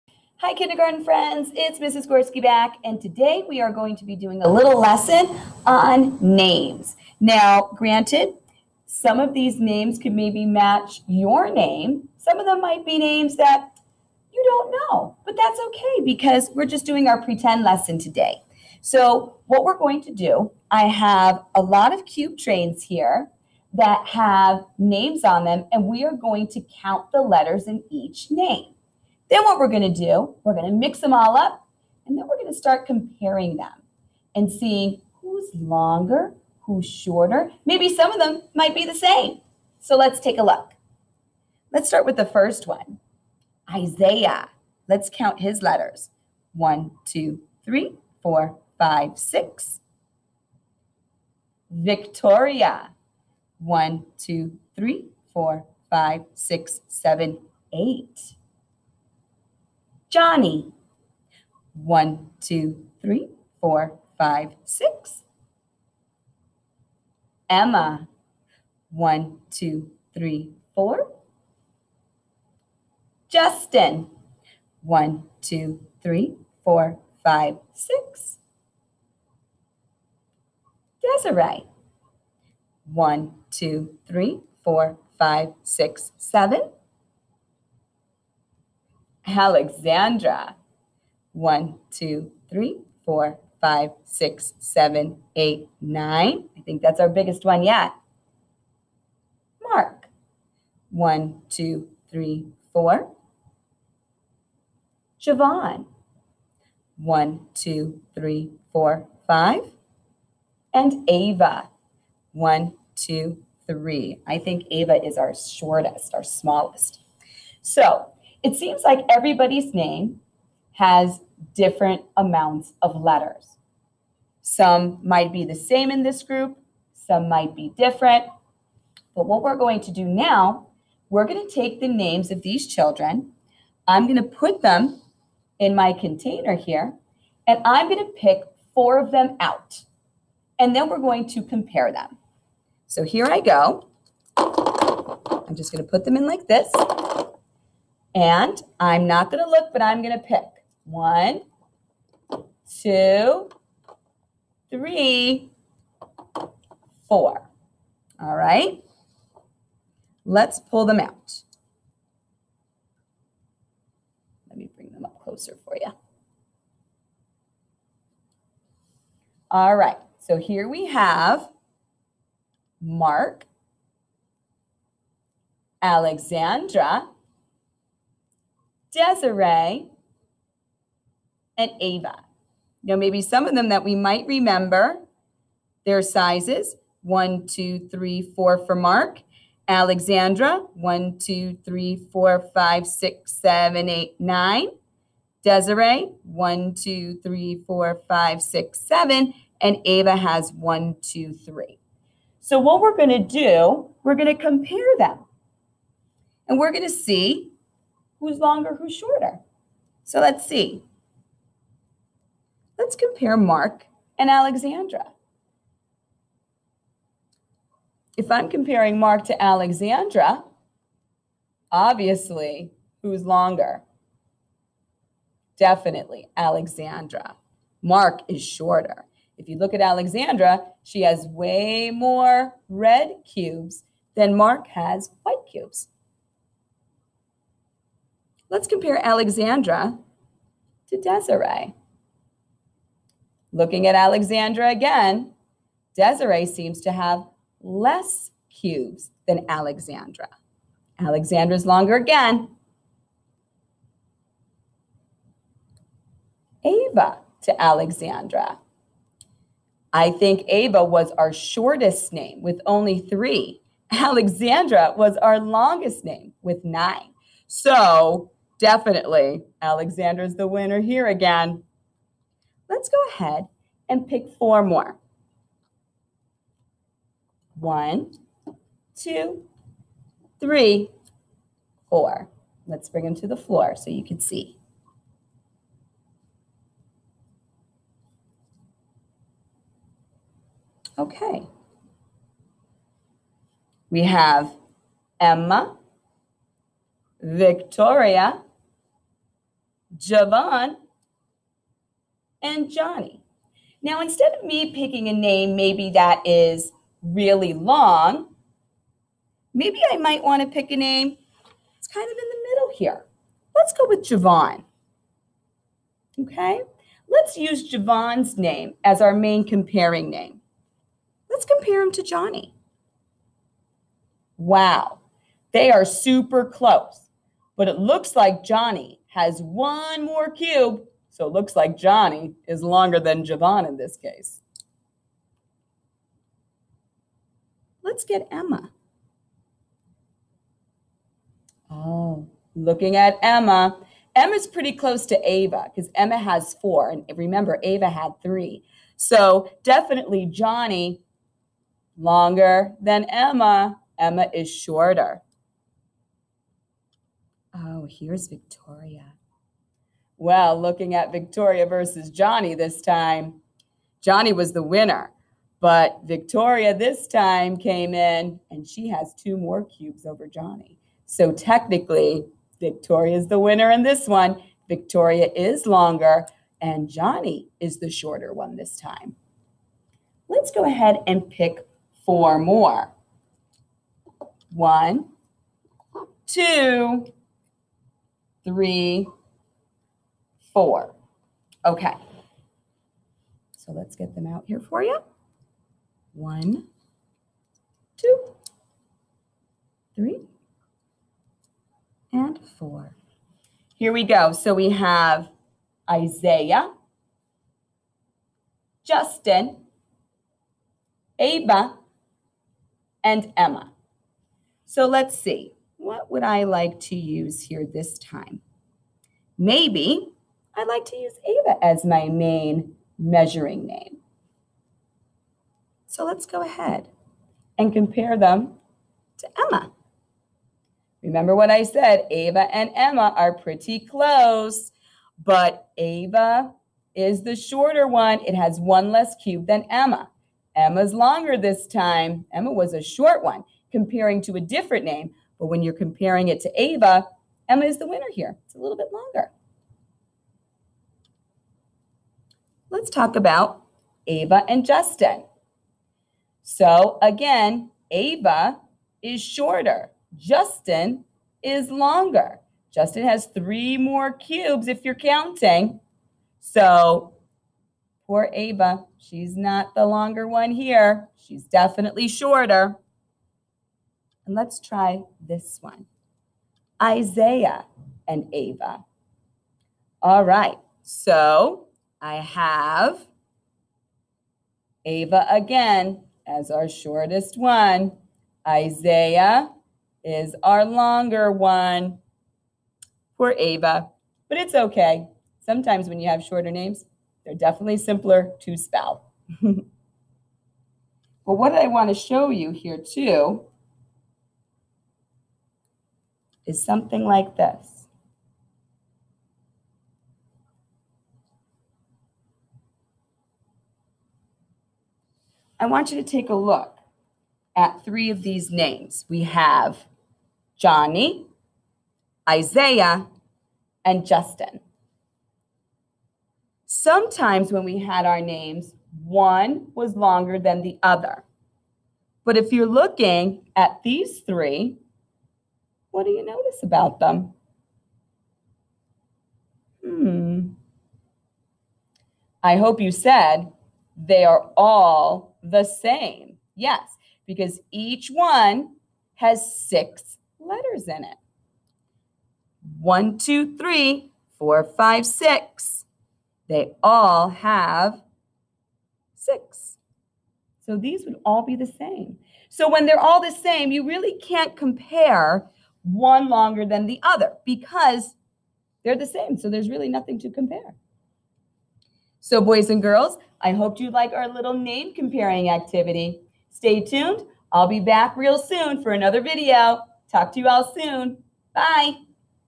Counting and Comparing Activity: Letters In Our Name/Comparing Names
This lesson is focusing on counting and comparing the number of letters in various names. This lesson is intended for early learners and Kindergarten students that use the math program, Investigations.